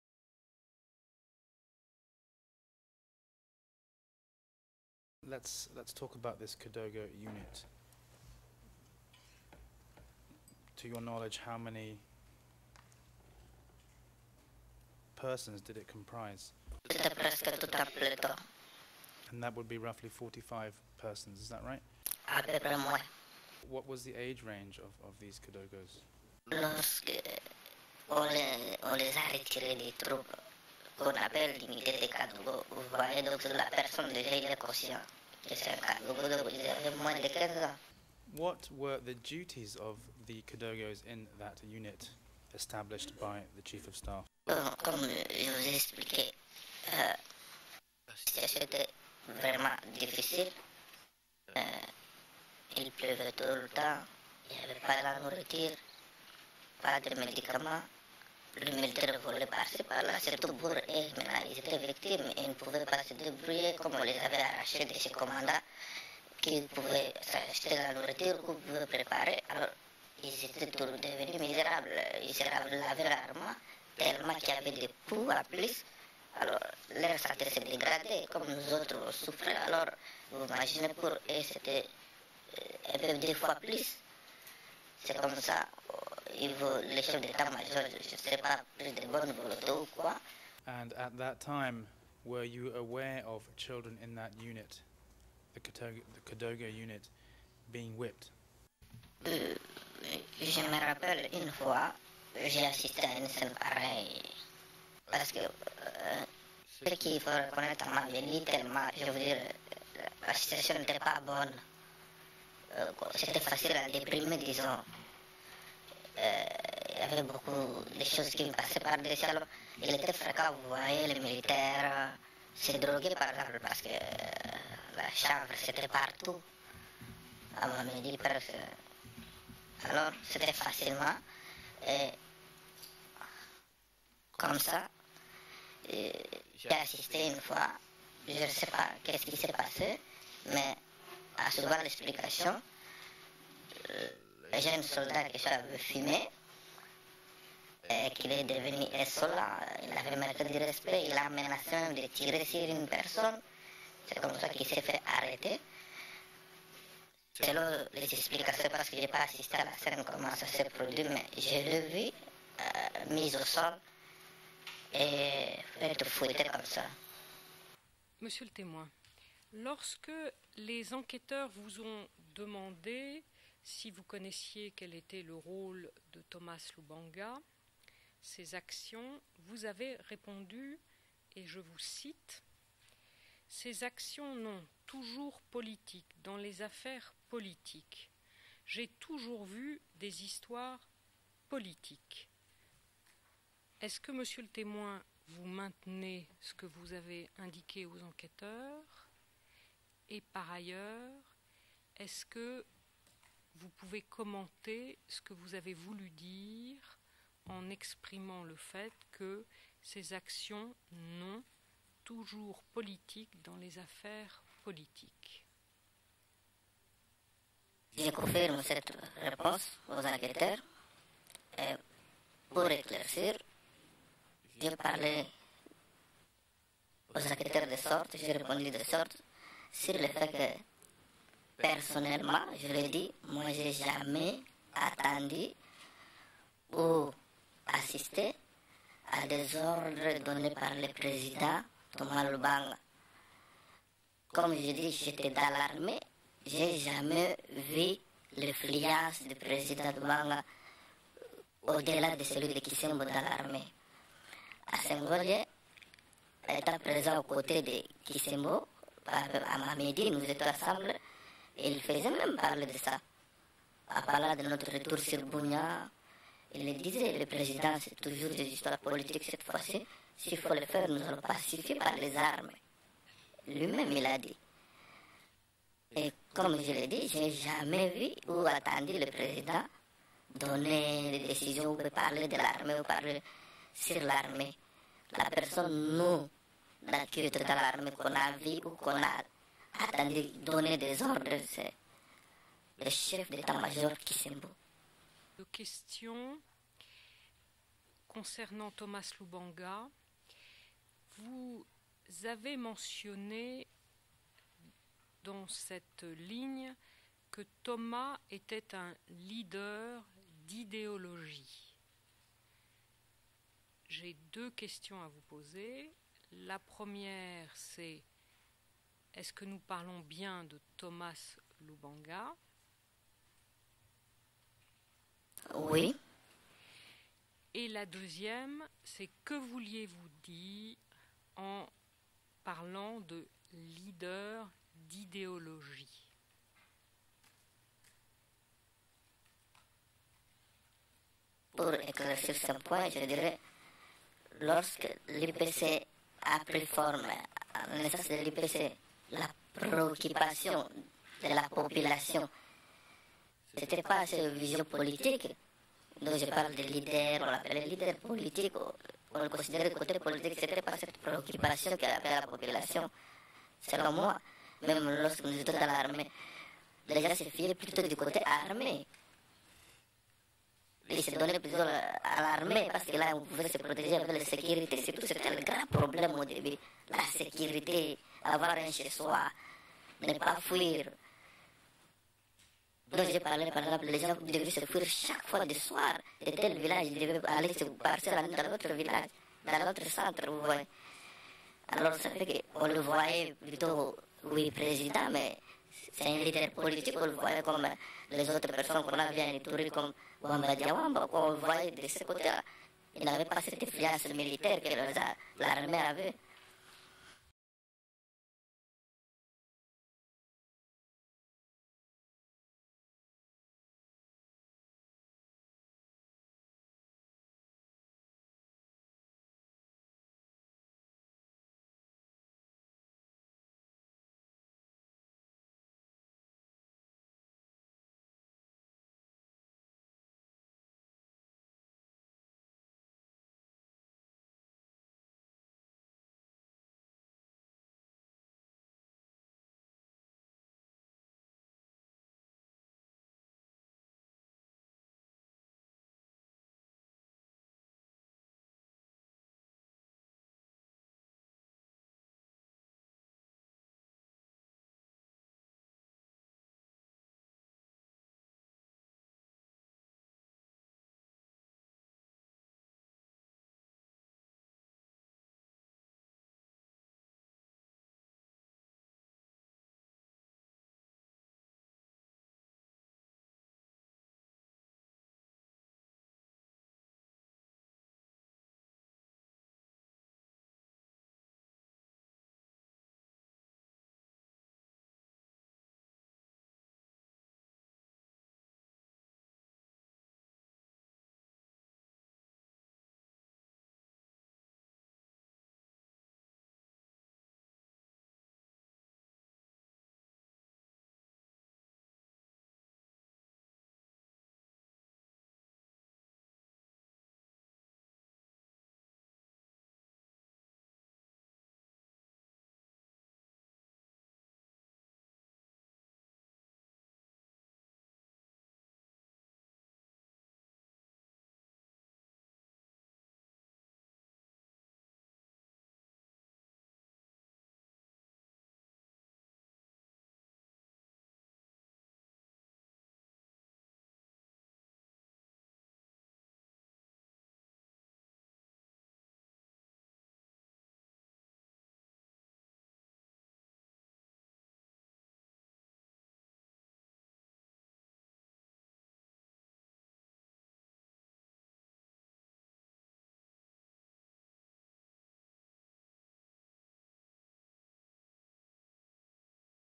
Video summaries from 3.30-4.3 of the Thomas Lubanga Dyilo trial
Weekly audio and video summaries of the hearings held between 30 March and 3 April 2009 in the case of The Prosecutor v. Thomas Lubanga Dyilo